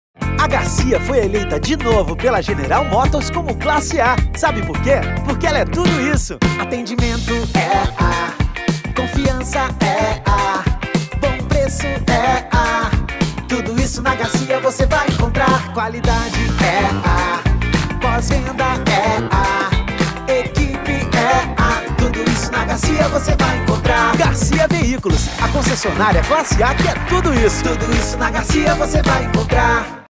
Concessionária Classe A - Garcia Veículos
A Garcia mais uma vez se firma como uma concessionária de qualidade ao conquistar, da General Motors do Brasil, o Título de Classe A pelo segundo ano consecutivo.